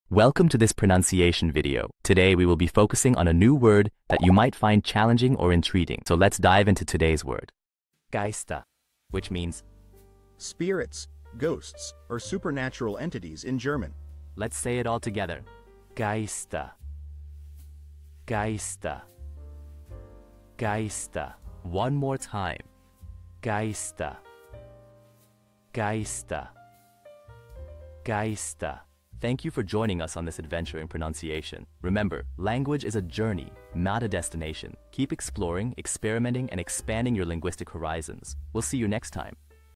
How to pronounce Geister
Master the Pronunciation of 'Geister which means spirits' - which means : Spirits, Ghosts, or Supernatural Entities in German. 📝 with @PronunciationGerman 🎓🗣️ - [Your Guide to German]

Learn to pronounce 'Geister' perfectly in German with our easy and engaging tutorial by @PronunciationGerman! 📖📽️ In this comprehensive video, we provide you with native speaker examples 🗣️👂 and phonetic breakdowns 🎵 to ensure you get the pronunciation just right.

How to say Ghost in German